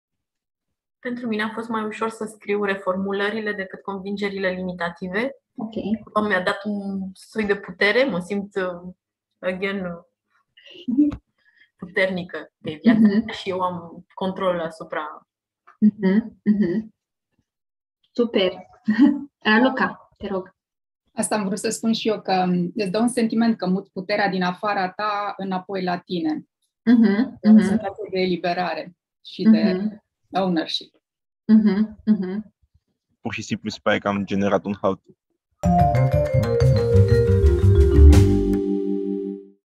Exercitiu demo Psychological Safety
Am testat in echipa unul din exercitiile practice din cadrul evenimentului "Psychological Safety Learning Day", un exercitiu de rescriere a convingerilor limitative. Iata rezultatele.